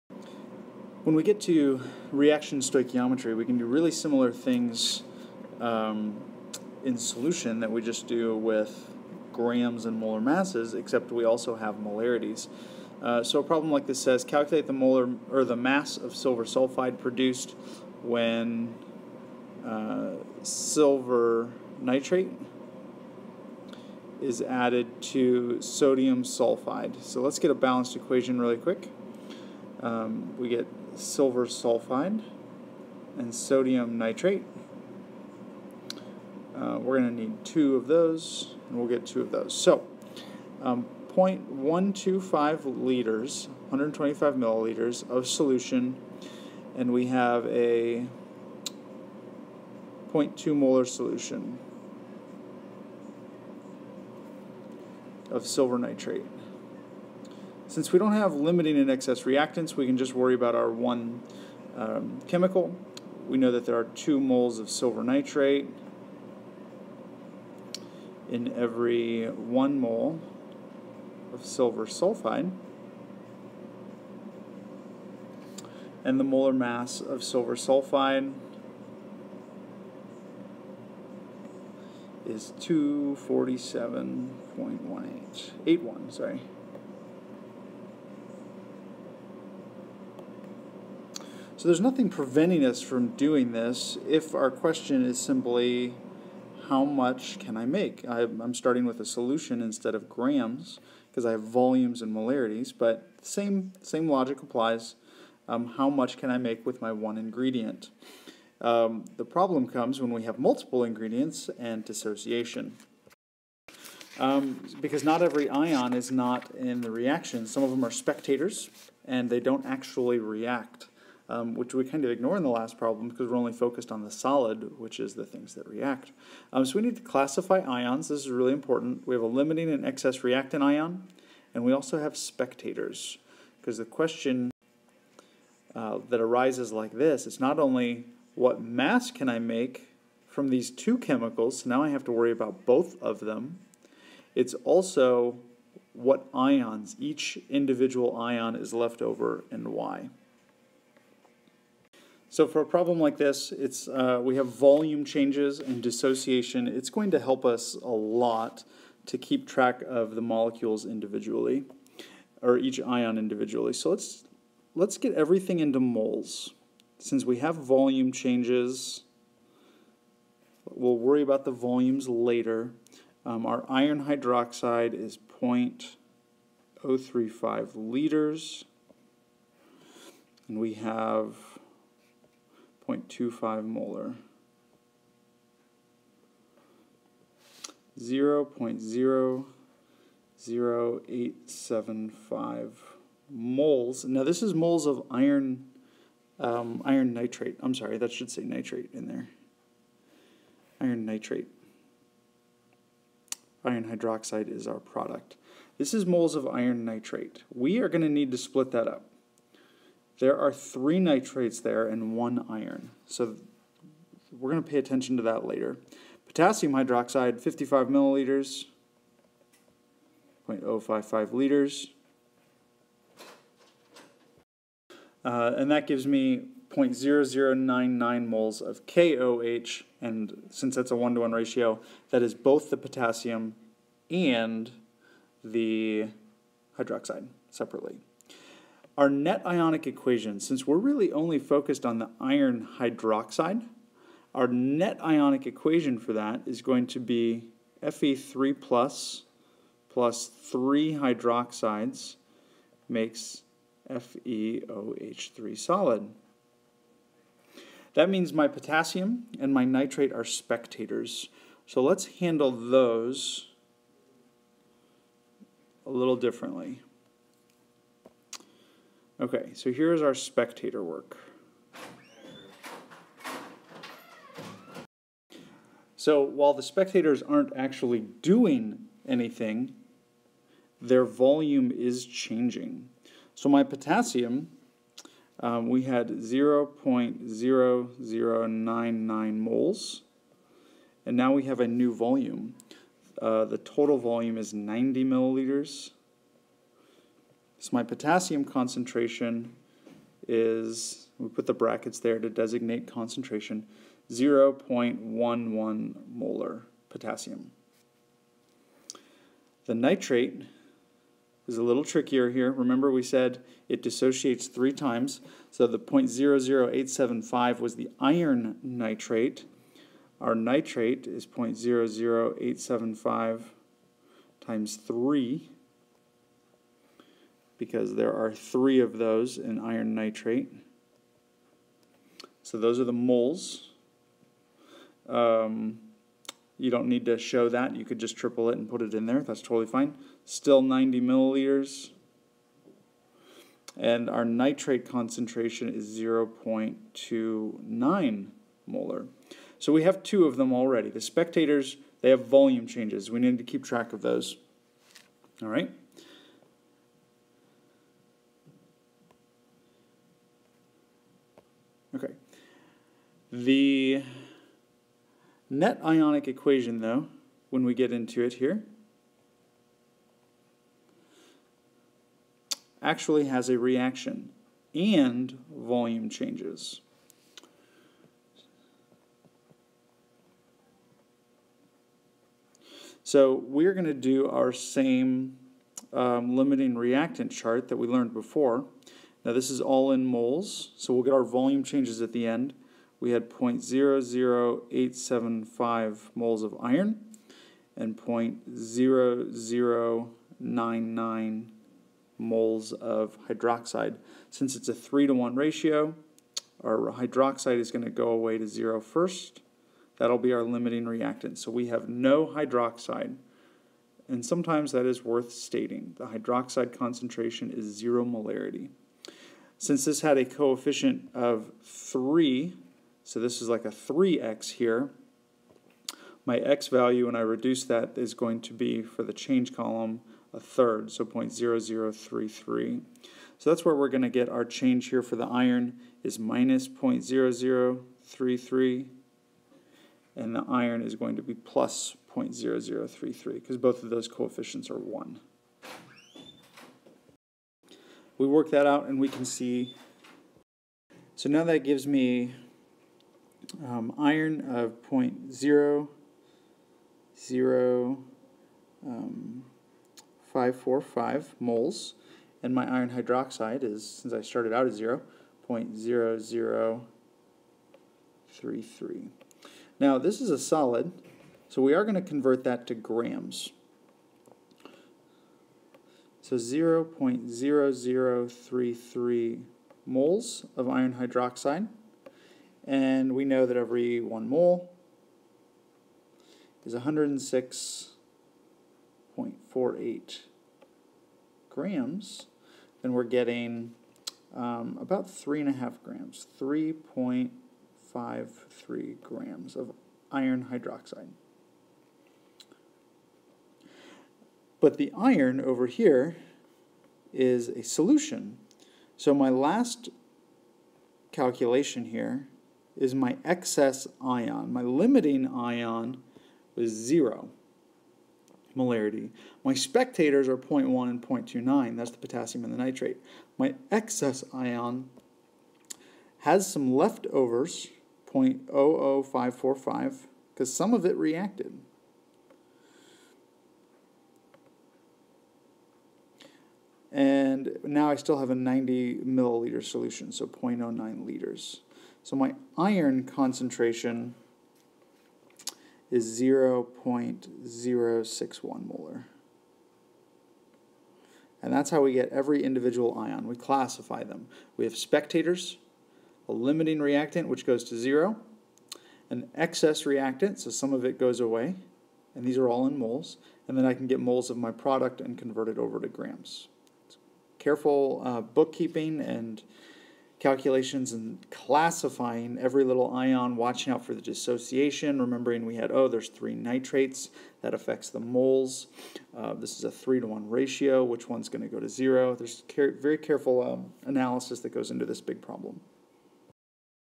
4.7
AP Chemistry Dublin Jerome High School

0:00 - 4.7 B Solution stoichiometry
2:00 4.7 C Solution ION concentrations